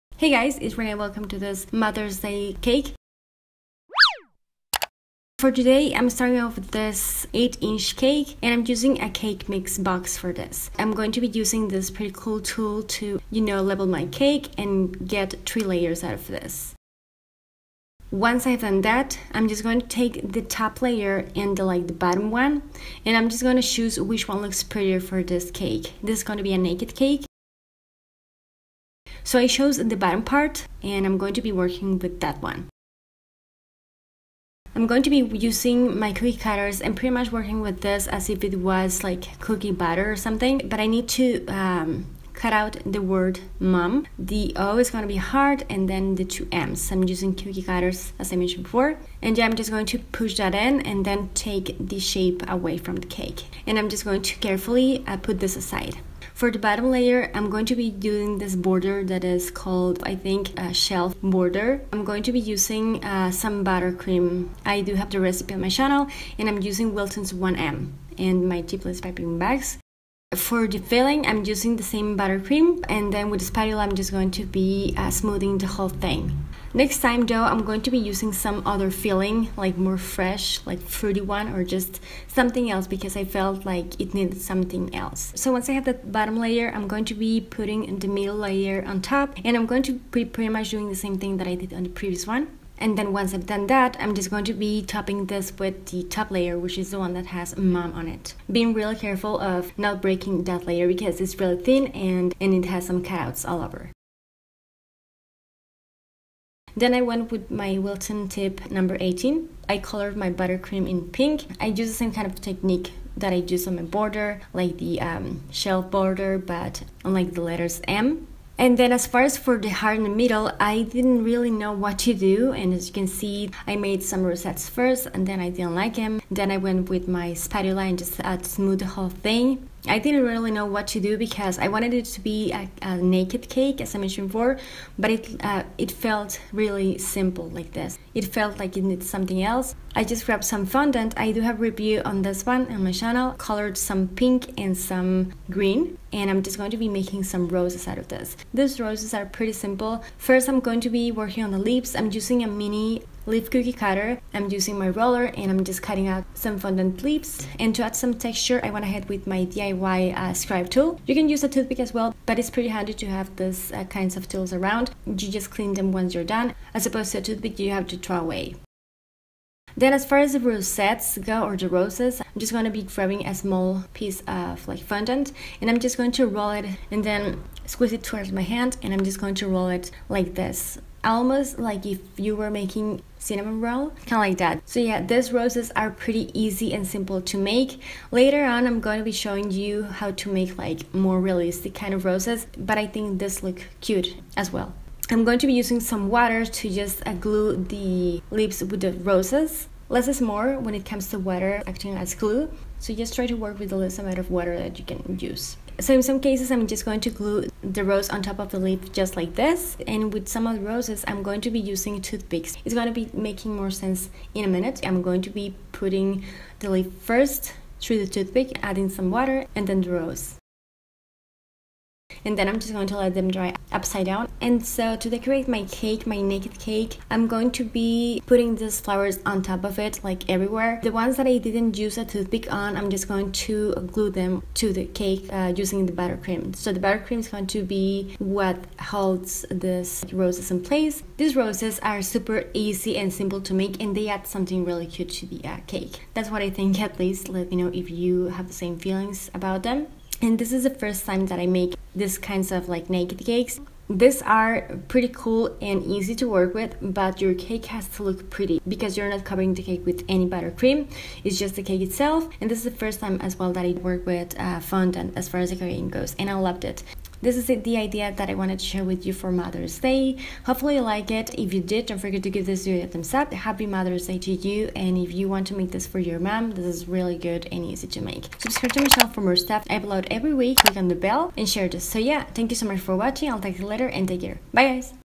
Naked Cake for Mother´s Day
Hi guys! I´ve seen naked cakes around for a while and I always thought they looked so cool, so finally I´m giving this a try for Mother´s Day, this also is the first time I´ve ever done anything with fondant on my channel, except for the fondant review I recently made, I loved both, I hope you like this cake too, and Happy Mother´s Day if you are a mom, you´re amazing!